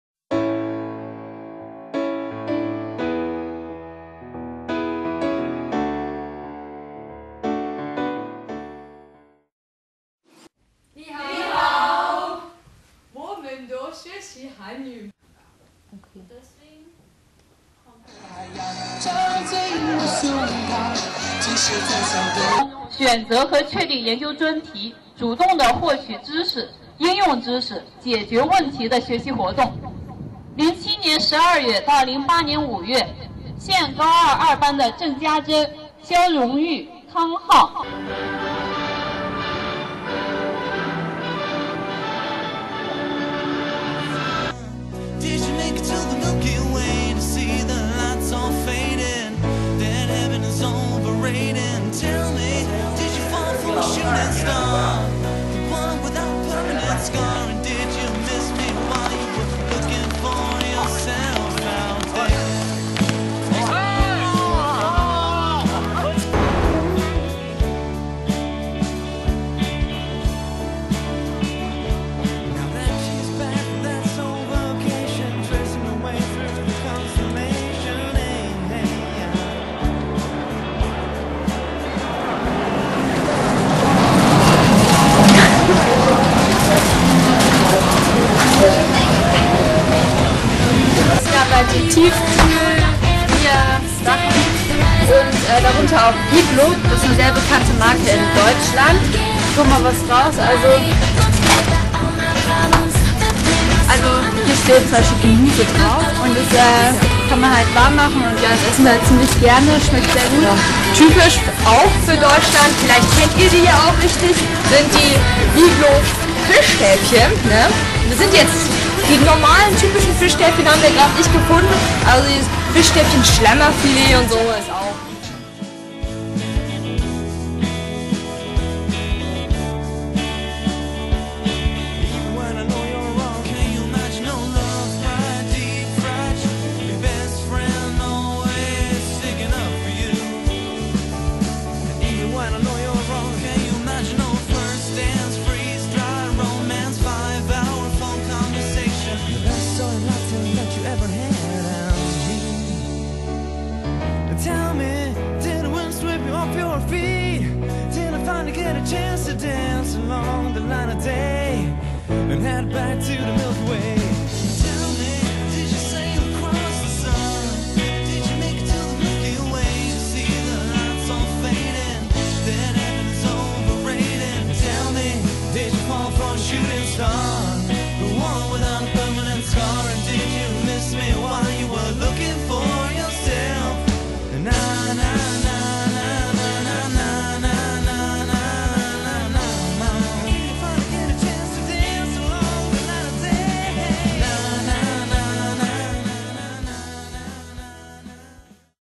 Deutsche Botschaftsschule Peking - Videobeitrag
Lebenswelten deutscher und chinesischer
10.-Klässler in Peking, ein Vergleich.